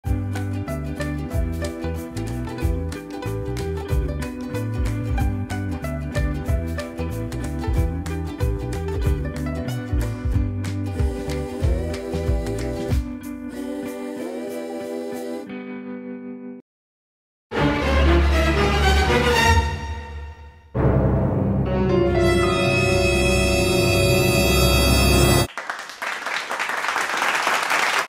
Kirjatraileri: Tuukka-Omar ja superkielitaito / Niina Hakalahti
Ymmerstan koulun neljäsluokkalaisten tekemä kirjatraileri. Ohjeet kirjatrailerin tekemiseen löytyvät täältä

Kuvalähde Pixabay.com ellei toisin mainittu.